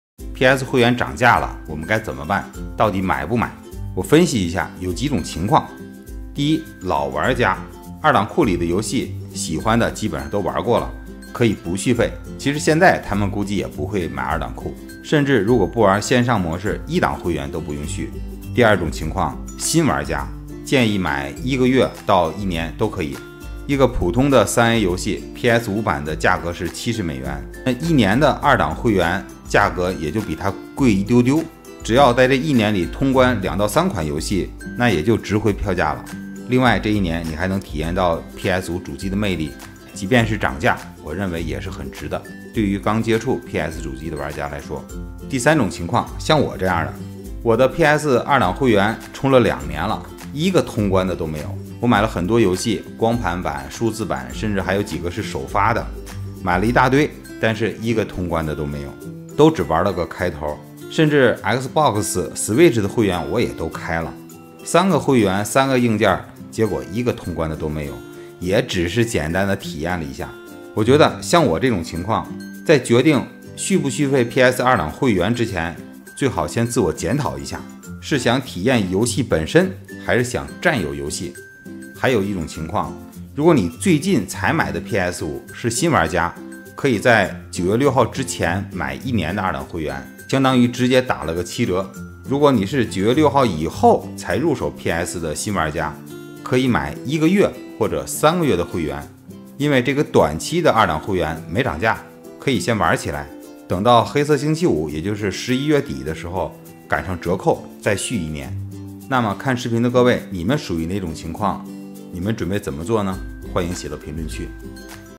索尼游戏PS+会员涨价后，你还会续费吗？什么时候续？分四种情况
面对索尼PS+会员涨价，不同玩家选择不同，9月6日之前续费？黑色星期五续费？充它个5年？